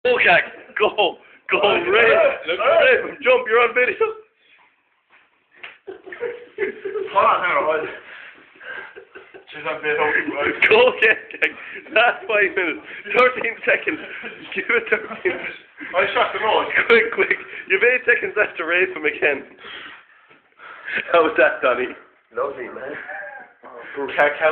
Young donnegan gets Hairy Aped!
Young red haired lad gets his ass pumped in his room by a sweaty corkman. Chief Suspect 'Khak'...